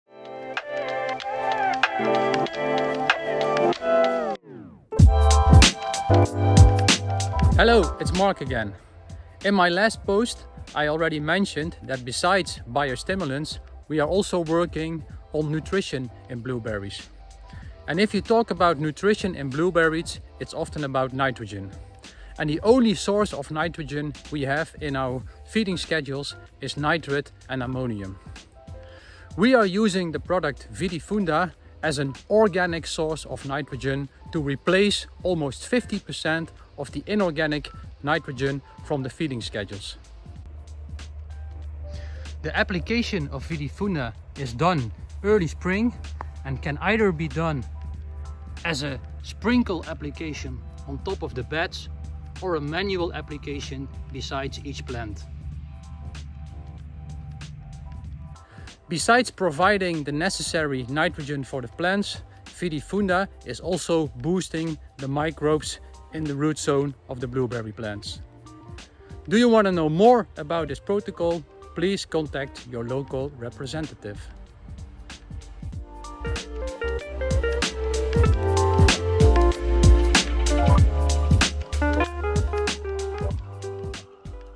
Using organic nutrition in blueberries by our consultant Mark van der Werf
How to give your blueberry plants more organic nutrition. Our consultant tells you more about how to do this.